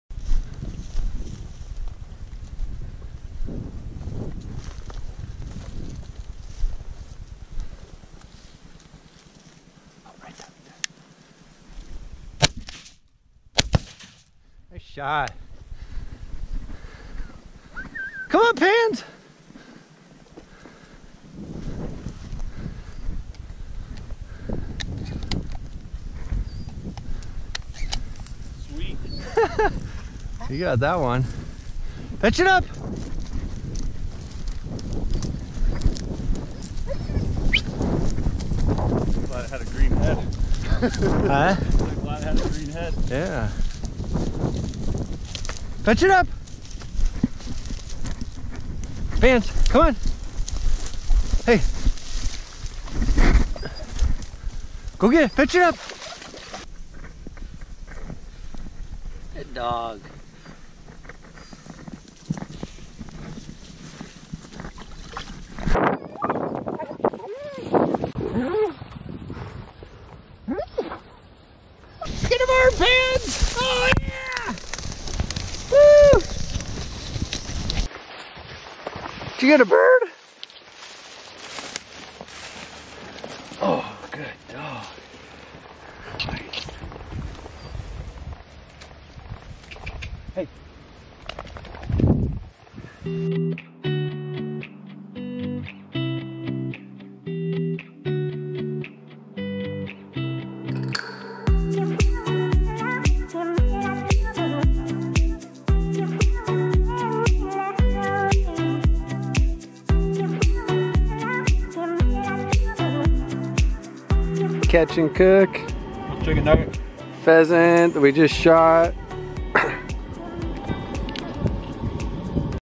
Jump Shooting Ducks and Pheasant Hunting
Some old footage I had out pheasant hunting in the Basin for pheasants. We managed to jump shoot a few ducks too, and had a clean and cook.